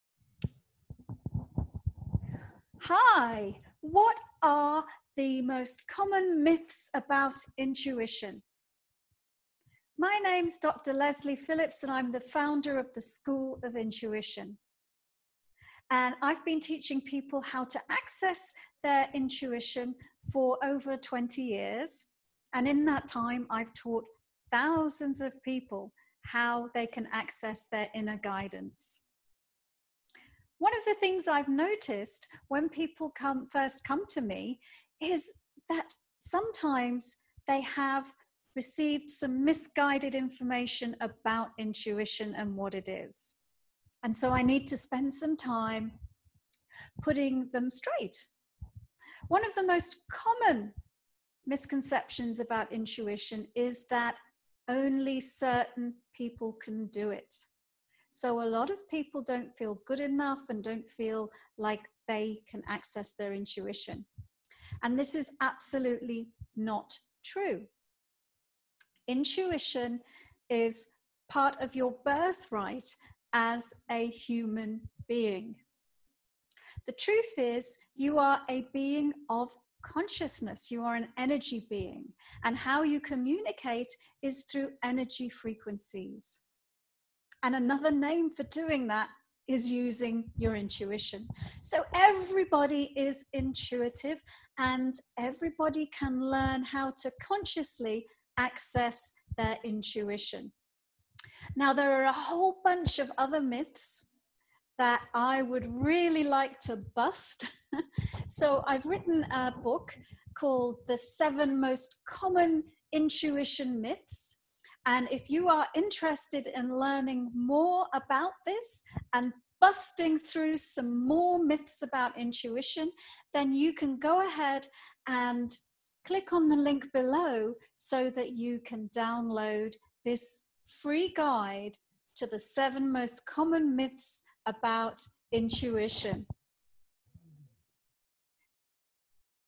The 7 Most Common Intuition Myths
What are the The 7 Most Common Intuition Myths
Fascinated by intuition? But not sure if you have the correct information? Dr. Lesley puts you straight.